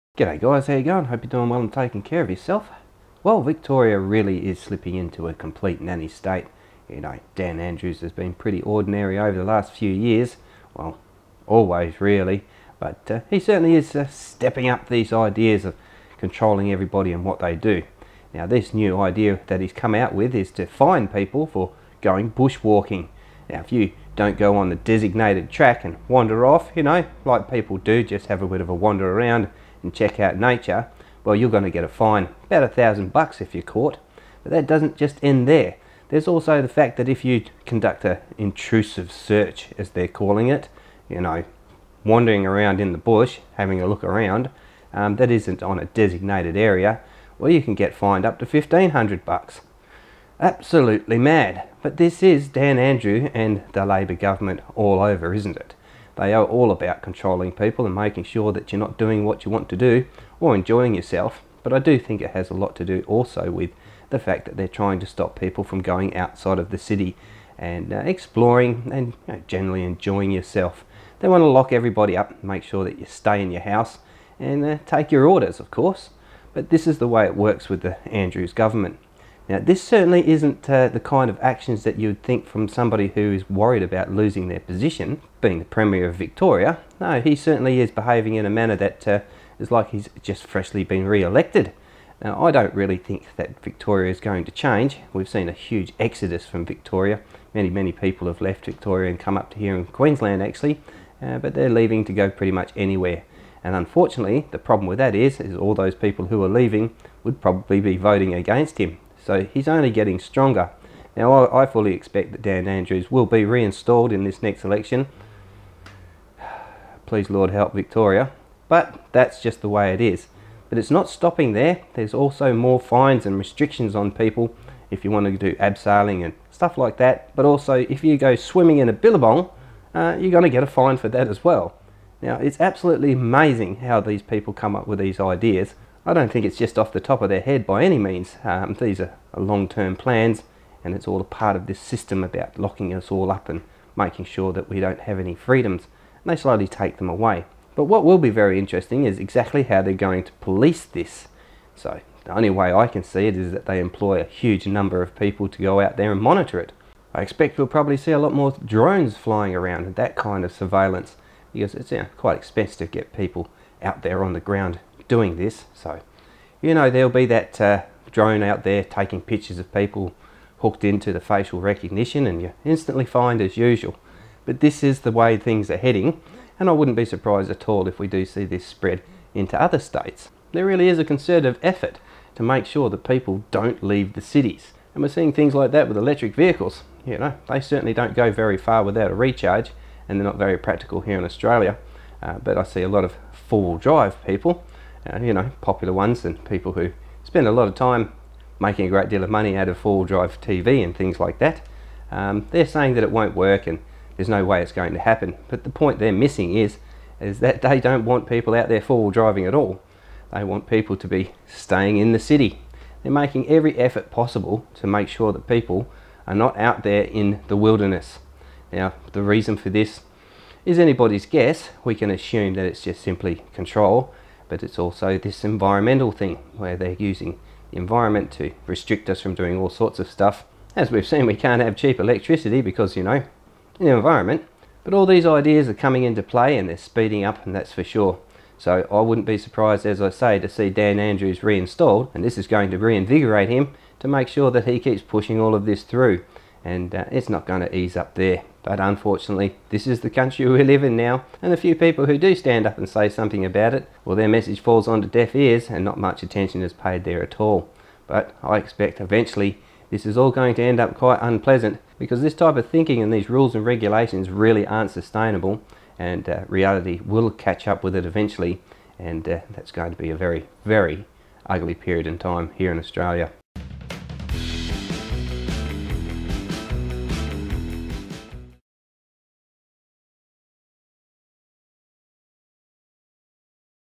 'Dictator Dan's' new nanny state rules
Daniel Andrews has been accused of trying to install “nanny state” rules across his state’s city and regional parks.

Walkers and riders caught not using government-sanctioned trails could face heavy fines, while swimmers could be barred from some waterways without a permit.

Help GTV continue to make videos you can donate using Paypal